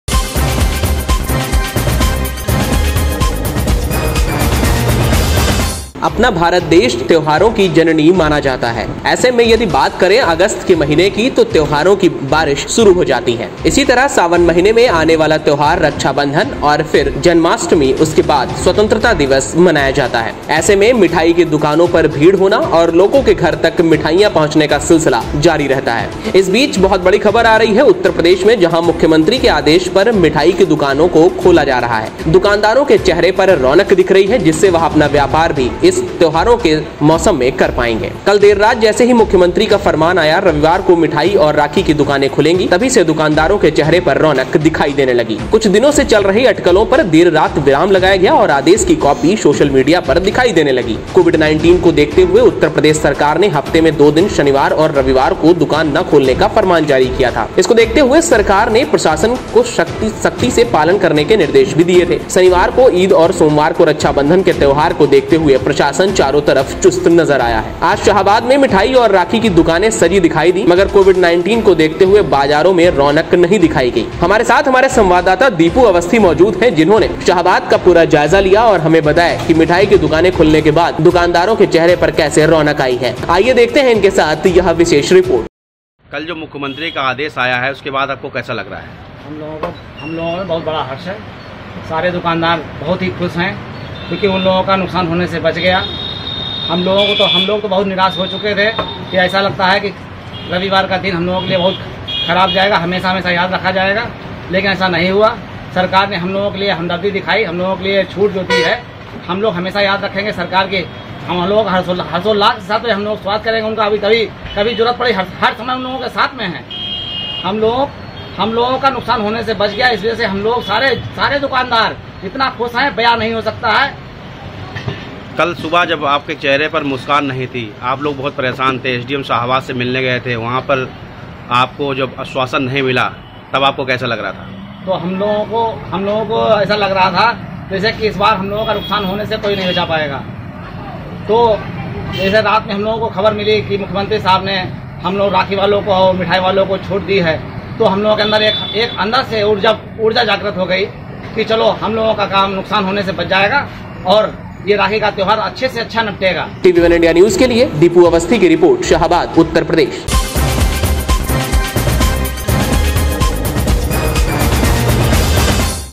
त्यौहार पर खुली दुकान तो व्यापारियों ने जताई ख़ुशी
दीपू अवस्थी,शाहाबाद

For Mobile App:.